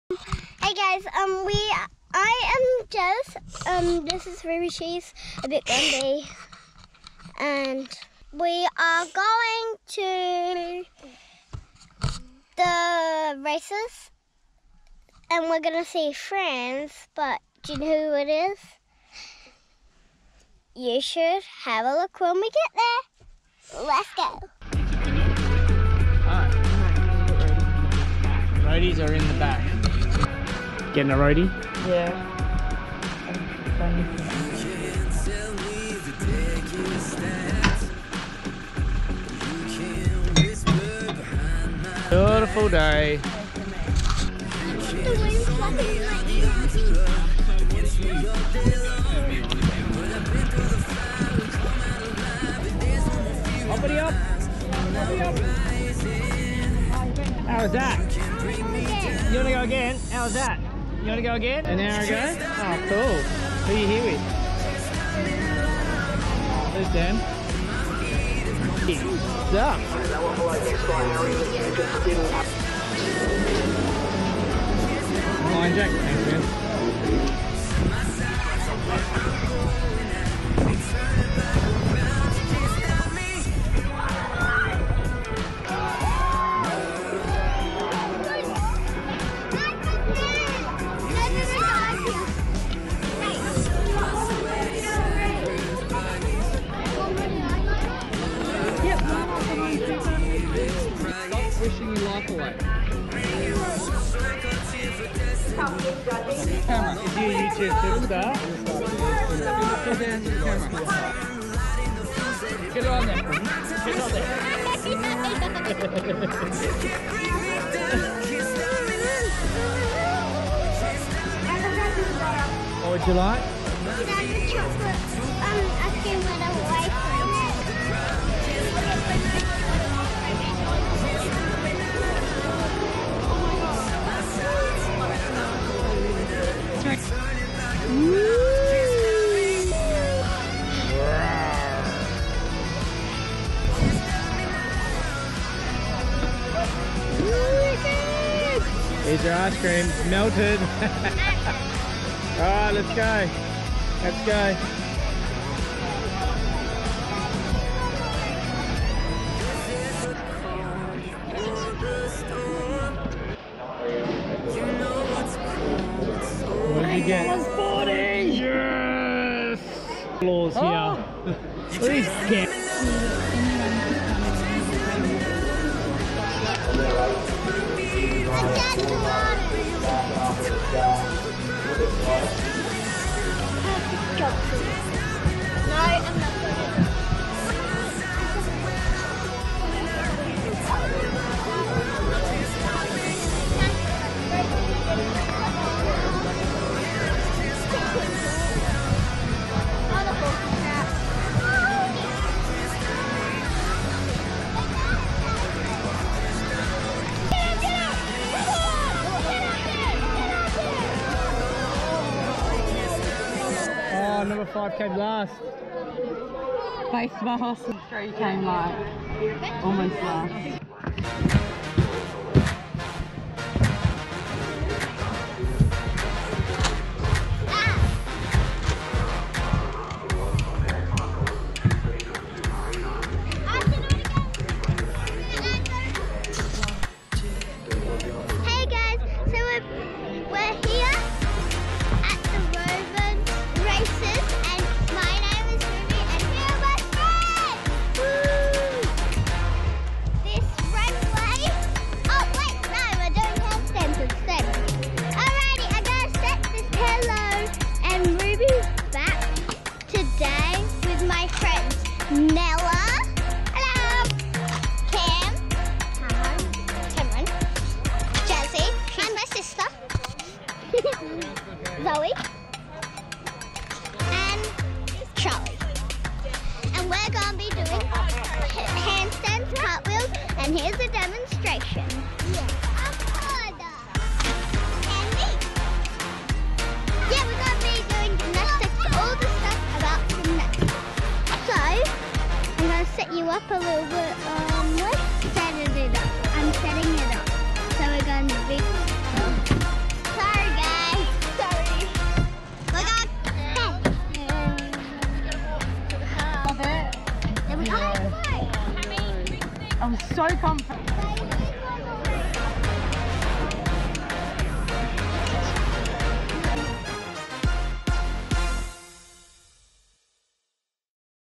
Family Day at the Nor West Jockey Club, Roebourne Ep5
A Great Day out at The Nor West Jockey Club for Family Day.